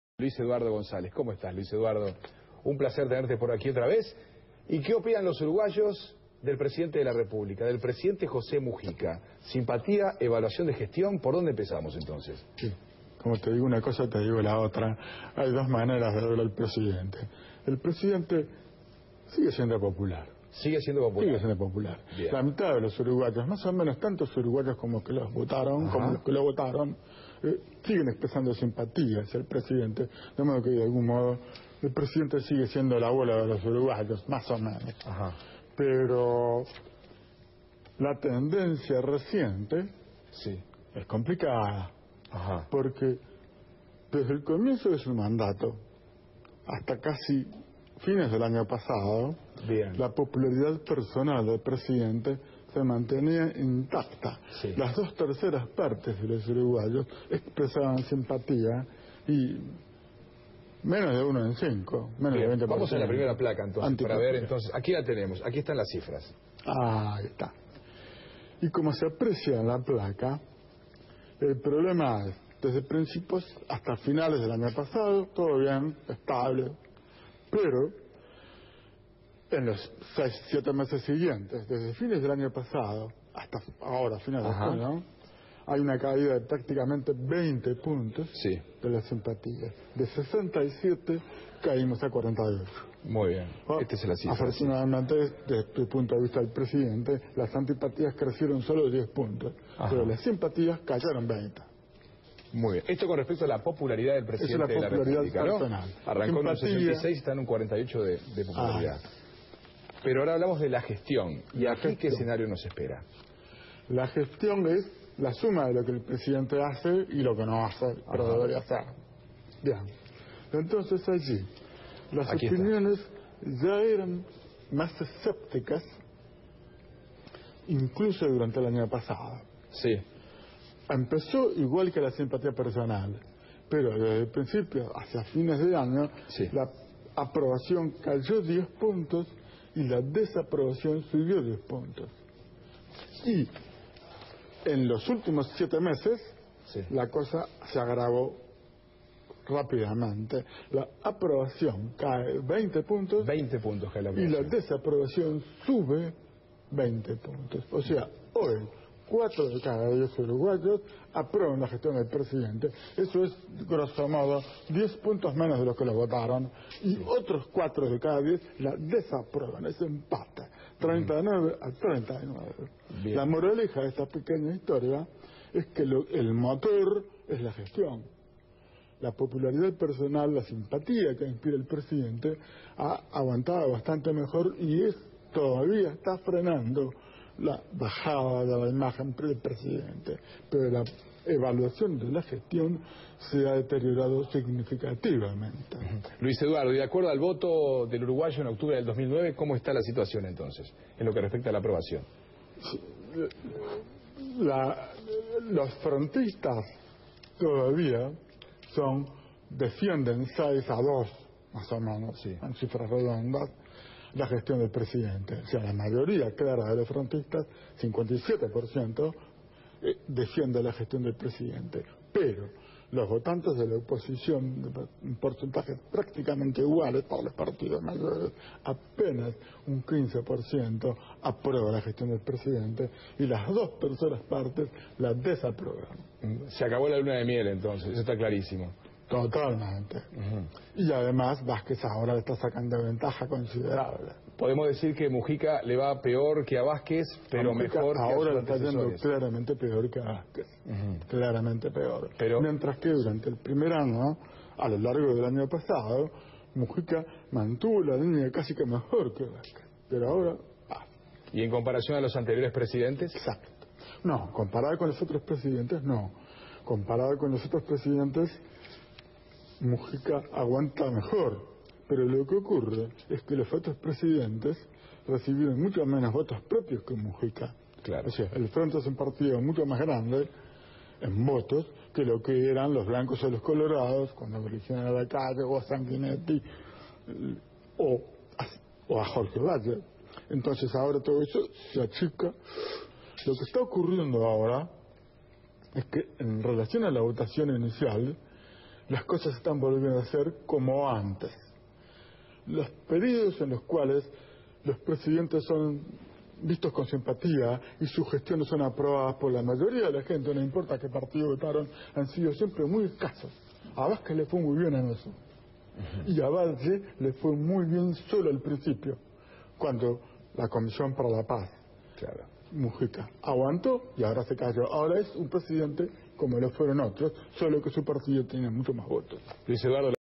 Mujica, gobierno y popularidad - Encuesta de Cifra 1º de julio 2011
Luis Eduardo González con la encuesta realizada por Cifra en cuanto a los niveles de popularidad y opinión de gestión efectuada a junio 2011